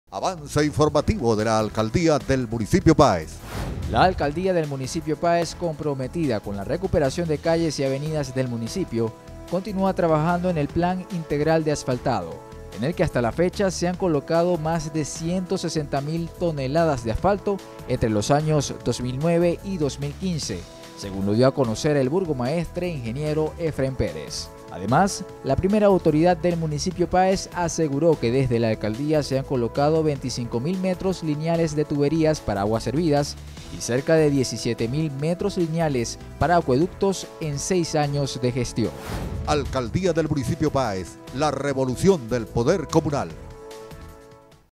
AvanceInformativo - Plan Integral de Asfaltado en Páez
Así avanza el Plan Integral de Asfaltado en Páez

Ya son más de 160 mil toneladas de asfalto colocadas en todo el municipio Páez desde 2009 al 2015. Cabe resaltar que esta cifra sigue en constante aumento.